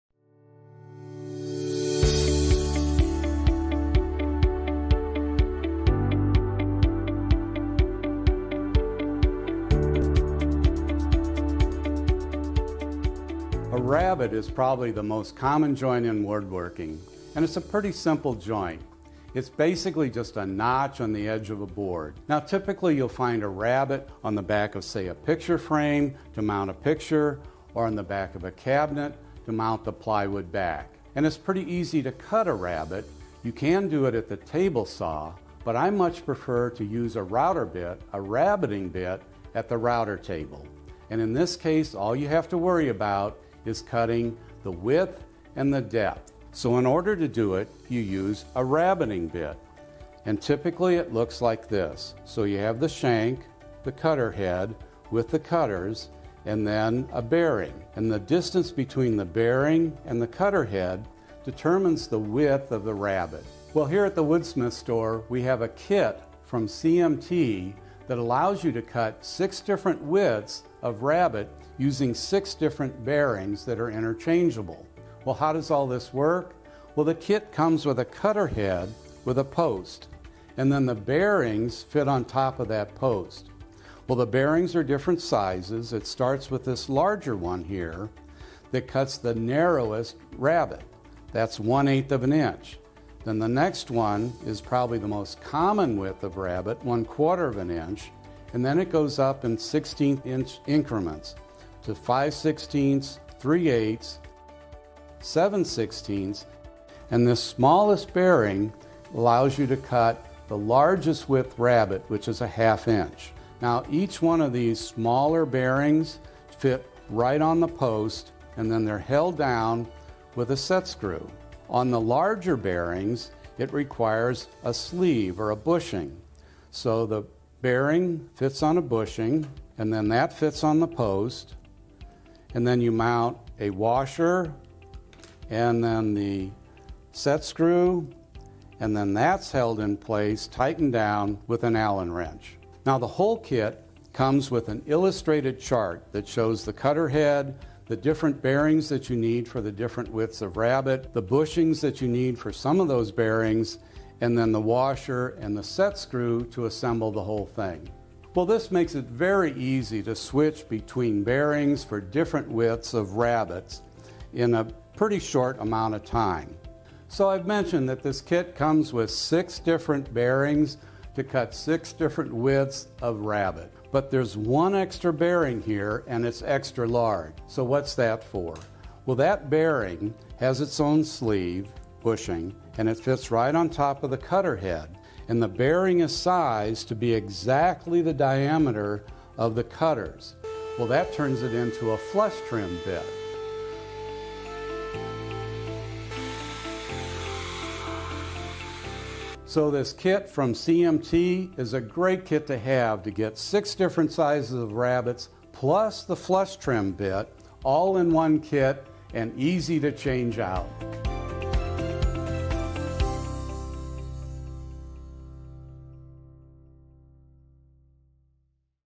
Cut 6 Rabbets + Flush Trim with Just One Router Bit
Cutting rabbets is now easier thanks to this NEW rabbeting bit. This 7-in-1 router bit set cuts multiple sizes, from 1/8” to 1/2”, but easily converts into a flush-trim bit.

Get more tips like these + $25 in FREE online coupons, good for online purchases.